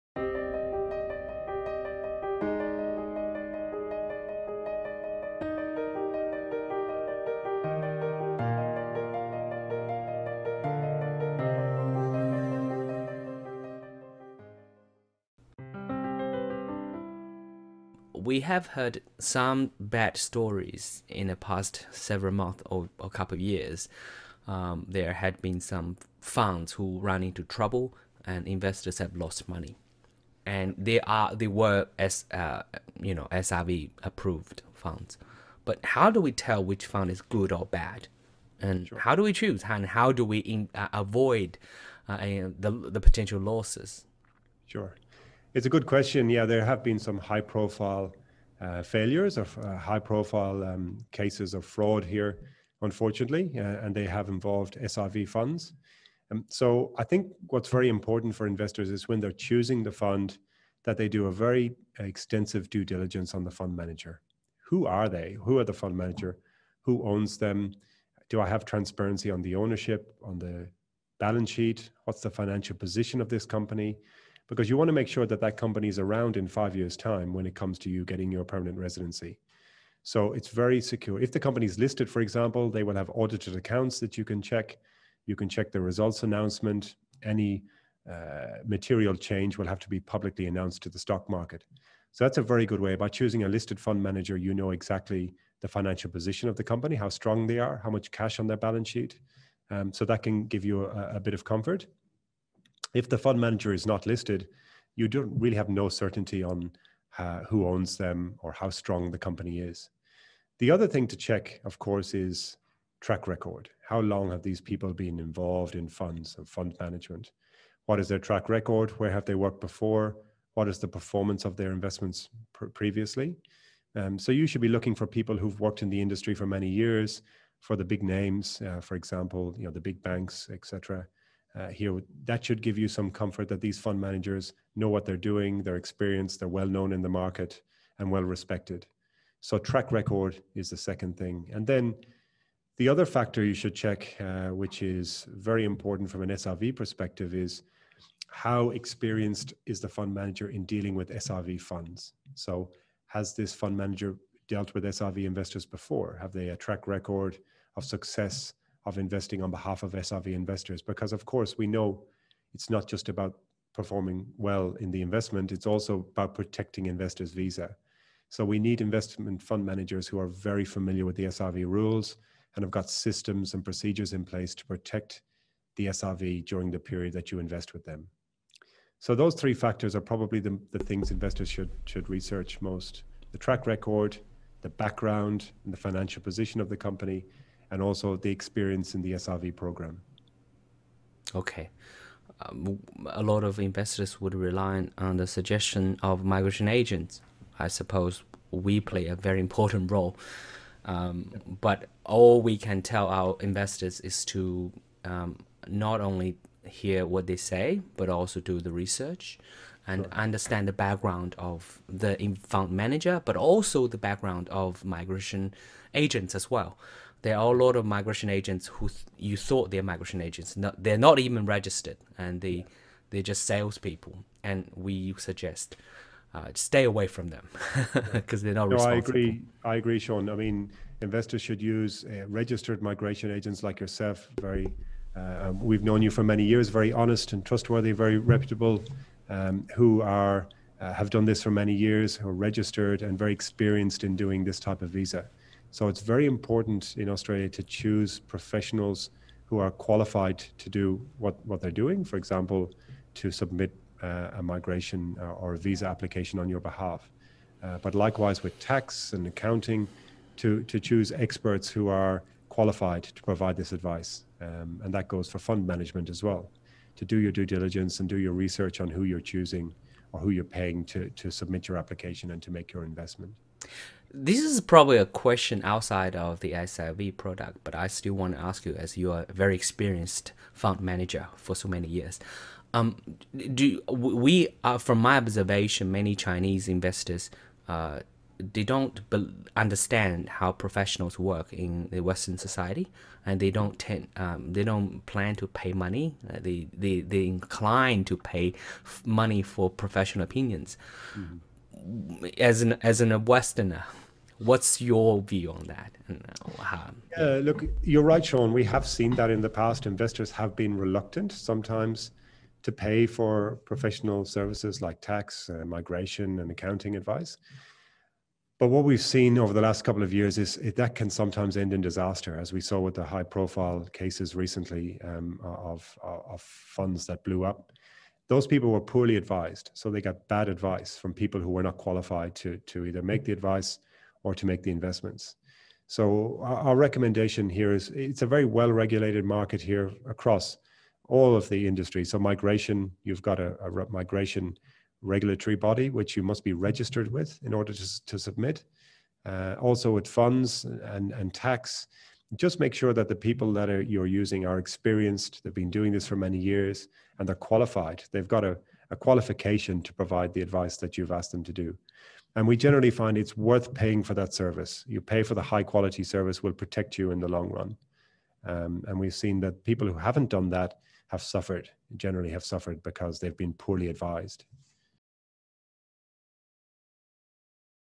澳洲投资移民防骗指南
大额投资移民投资产品诈骗在澳洲严格的监管机制下仍屡有发生，2020年最为轰动的莫过于地产投资项目iProsperity，其管理人Michael Gu躲避监管，私自挪用投资金额，导致数十名大额移民申请人损失本金，签证申请受阻。